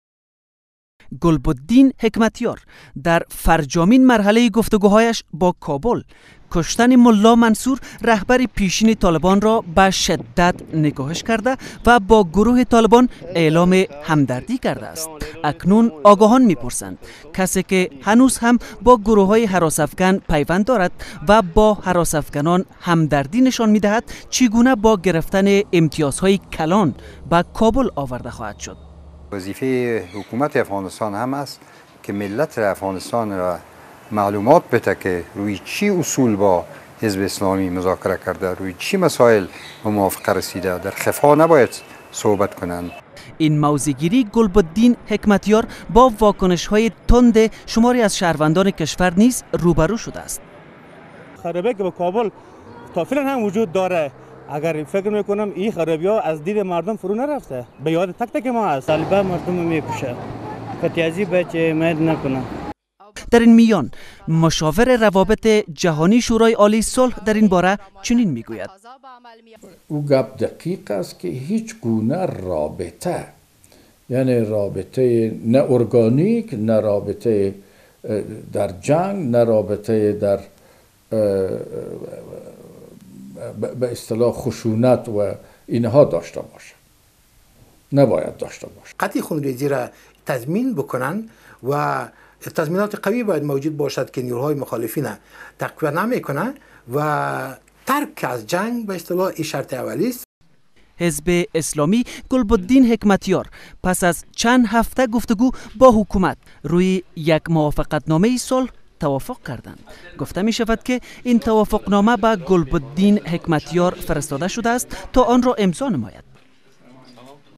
Analysts Negate Hekmatyar’s Remarks on Taliban Leader
واکنش‌های شهروندان کشور در برابر گفته‌های حکمتیار